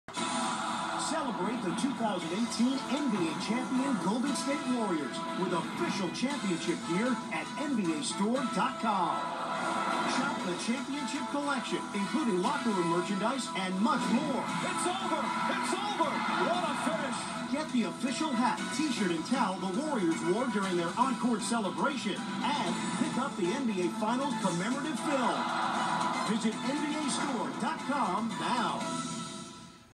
2018 Golden State Warriors NBA champs gear commercial
The Golden State Warriors won the sixth NBA championship title, over the Cleveland Cavaliers, 4 to 0 in the 2018 NBA Finals, presented by YouTube TV.
Featuring merchandise from the 2017-18 NBA season.
Taken during the postgame show of Game 4 from Quicken Loans Arena in Cleveland, Ohio on Friday, June 8, 2018 around 8:35pm on KABC-TV 7 Los Angeles, California.